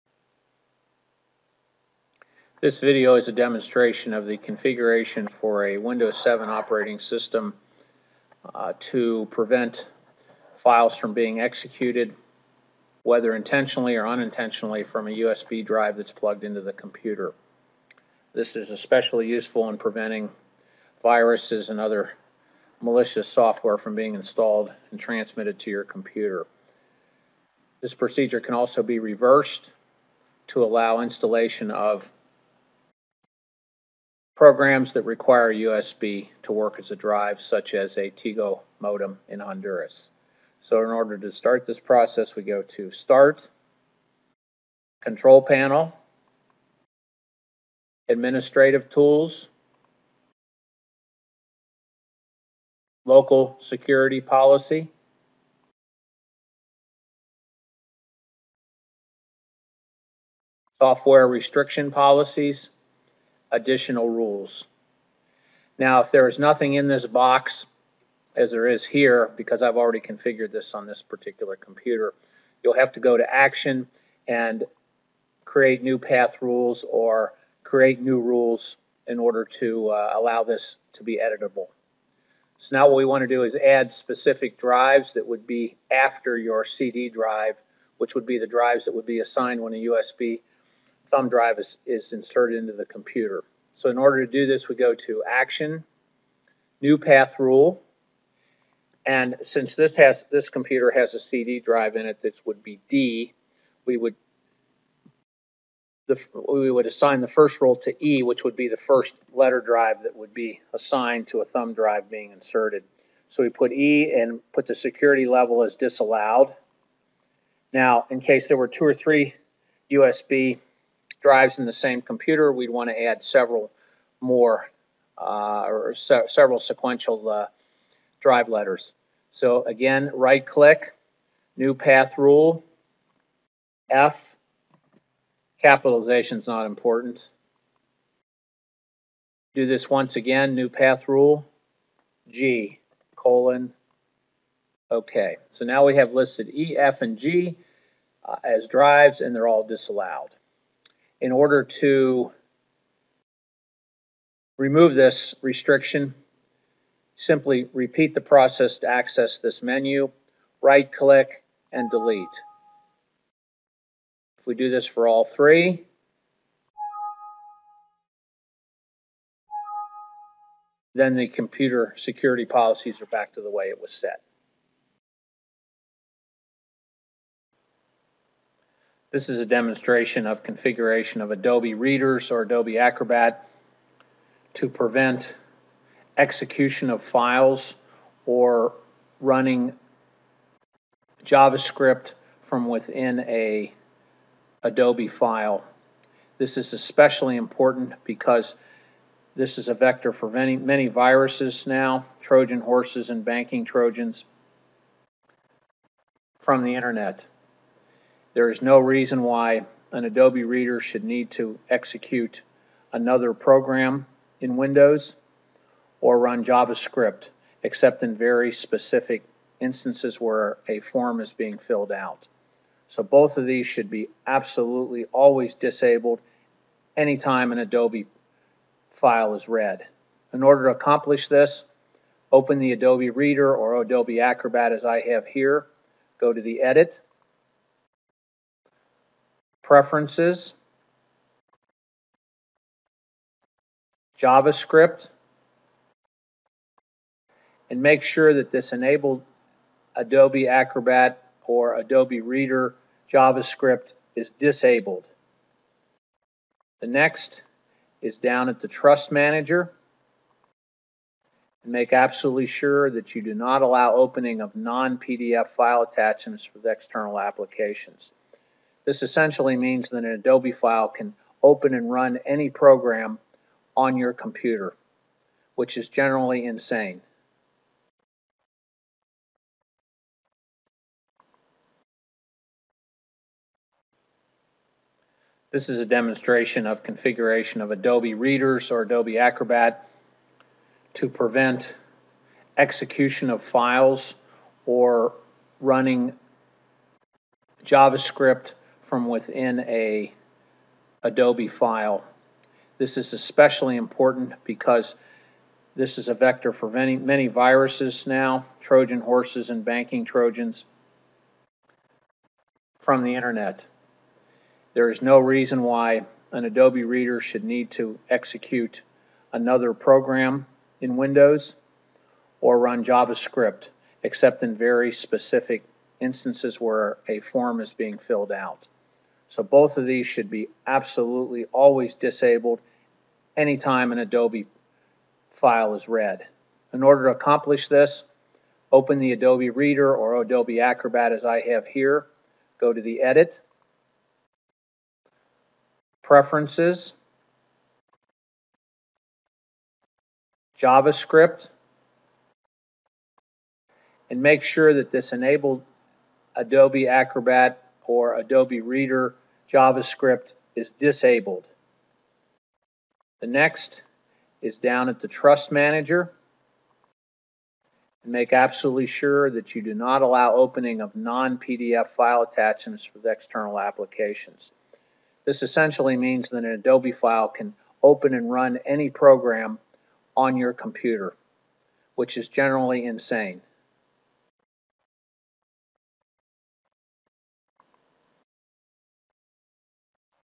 USB Lockdown.mp4
how to prevent execution of files from a USB drive plugged into your computer.